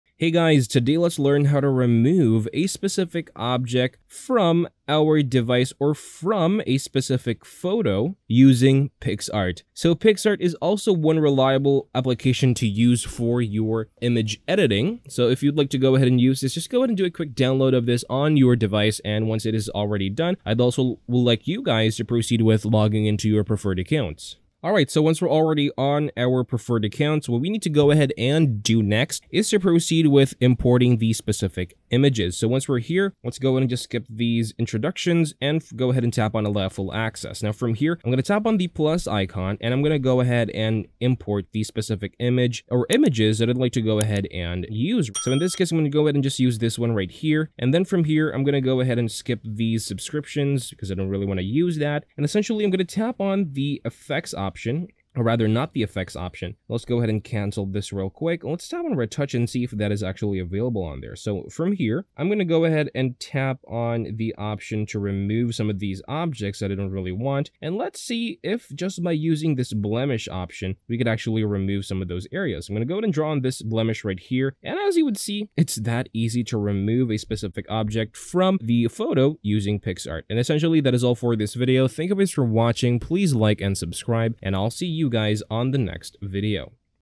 How To Remove Object From Photos In PicsArt (Guided Tutorial)
In this video, you will learn how to remove unwanted objects from photos with picsart easily. Stay till the end of the video to find out how to remove unwanted objects from a photo with picsart.

Did you learn how to remove objects from photos in picsart?

Do you want to see another video about how to remove objects from photos using picsart?

Did you enjoy How To Remove Object From Photos In PicsArt (Guided Tutorial)?